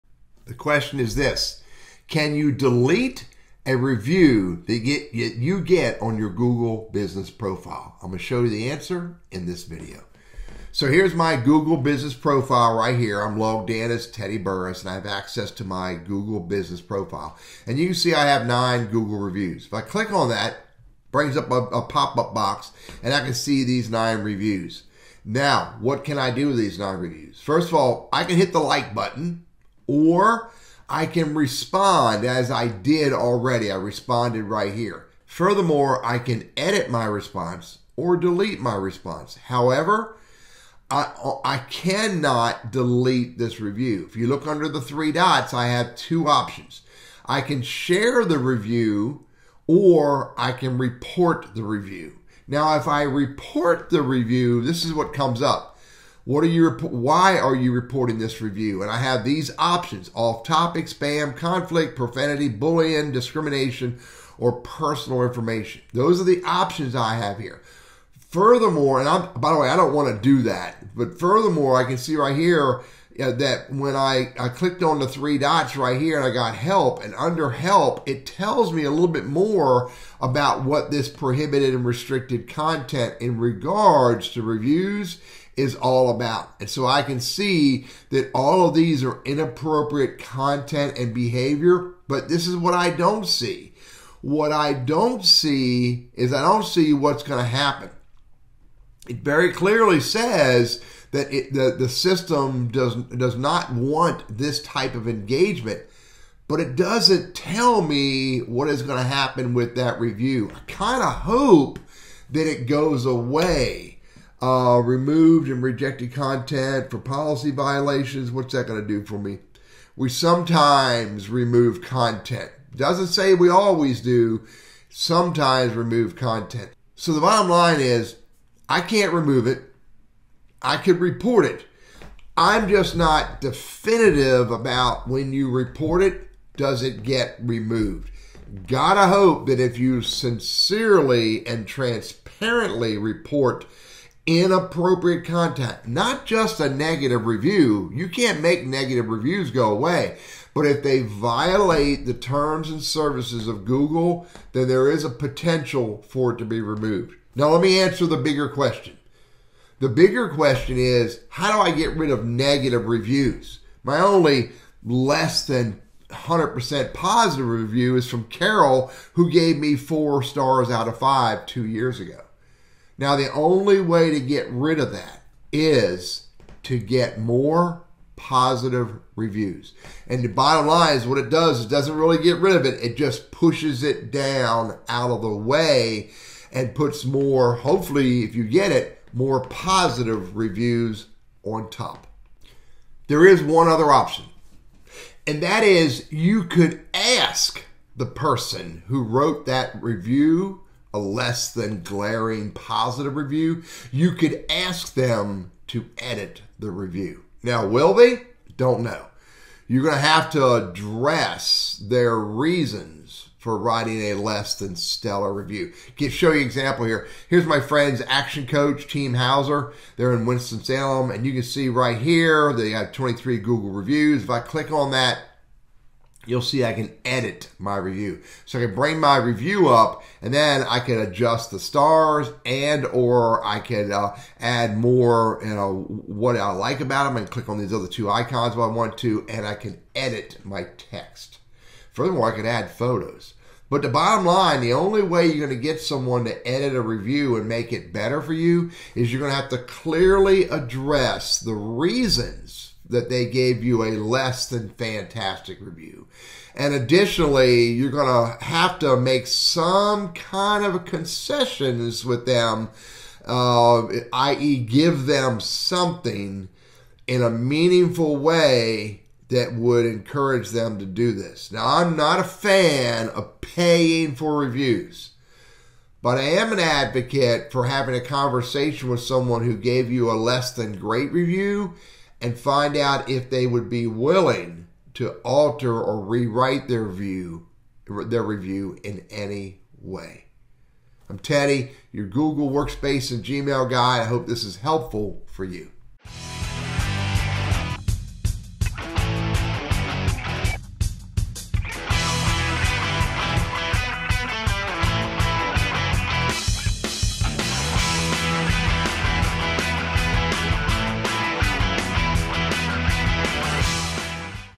How do I remove Bad Reviews on my Google Business Profile?
You can't remove a bad Review from your Google Business Profile.
You may be able to ask the reviewer to reconsider their review or delete it themselves.